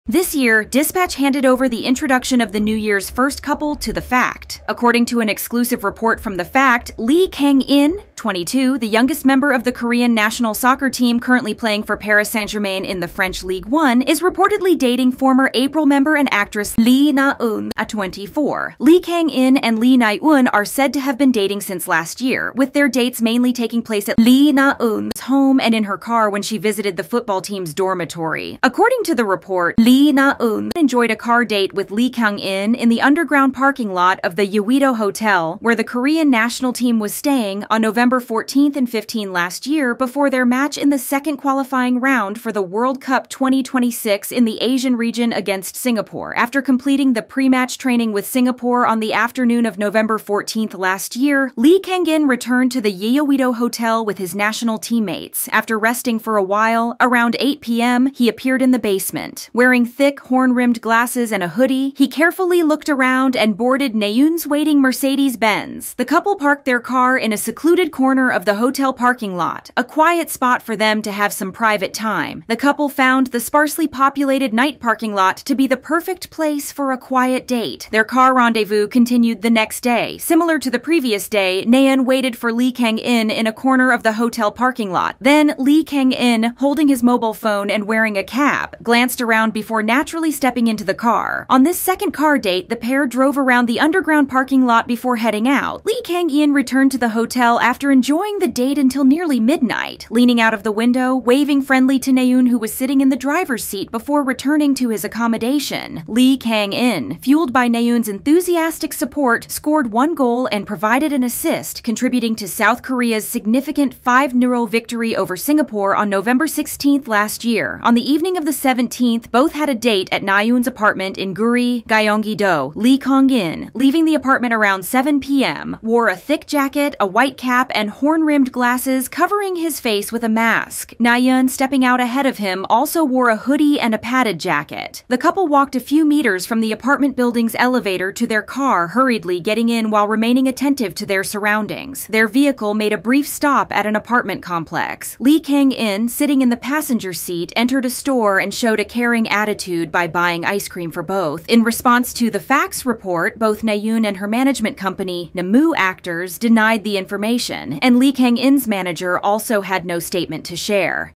New Year (2024) Couple: Former April member Naeun DATING winger Lee Kang-in of National Soccer Team
New Year (2024) Couple: According to an exclusive report from The Fact, Lee Kang-in (22), the youngest member of the Korean national soccer team currently playing for Paris Saint-Germain in the French Ligue 1, is reportedly dating former April member and actress Lee Naeun (24).